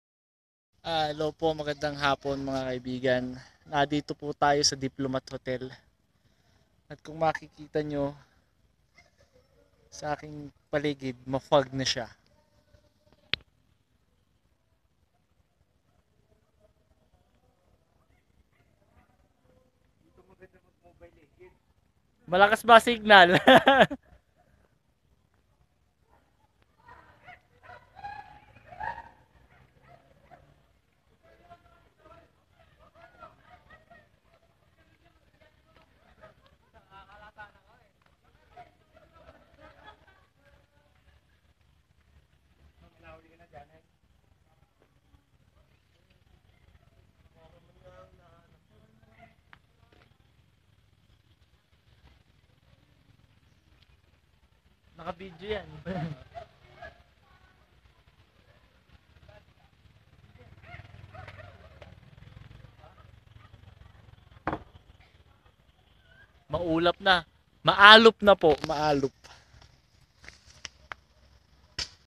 Diplomat Hotel
Video taken August 6, 2017.
Exploring the area room by room, floor by floor.